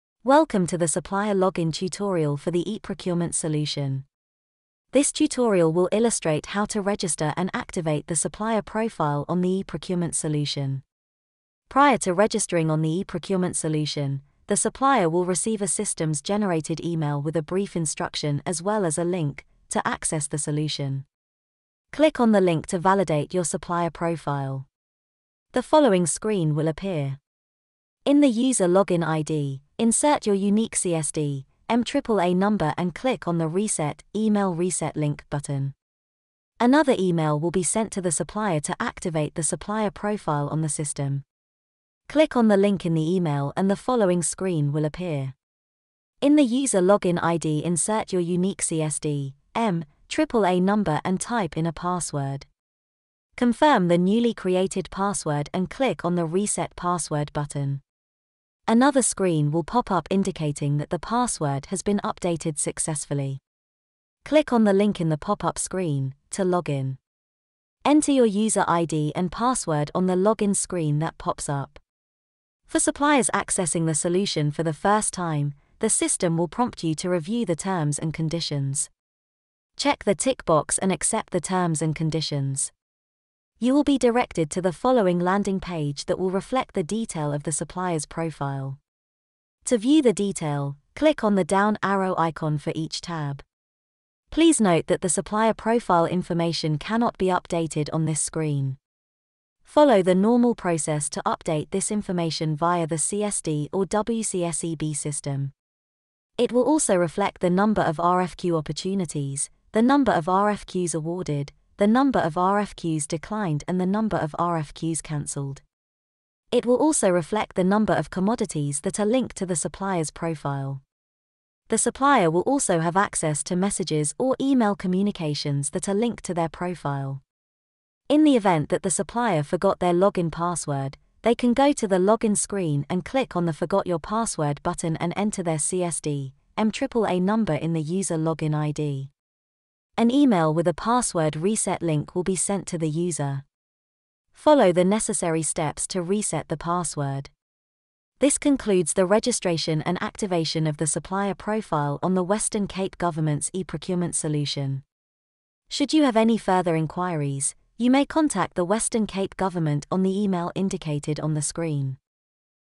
E-Procurement Solution: Video tutorial - supplier registration and profile activation
The current e-Procurement Solution known as the Integrated Procurement Solution (IPS) has been in operation since 1999 and facilitates the procurement of goods and services up to a certain threshold value. Given the recent challenges, the Western Cape Provincial Treasury took a decision to have an in-house solution developed to mitigate the various challenges experienced. In so doing, part of the implementation of the new solution requires that suppliers be on-boarded onto the new platform and subsequently activating their profiles on the new solution. The following video provides a step-by-step process to register and activate a supplier's profile, once in receipt of the system generated email.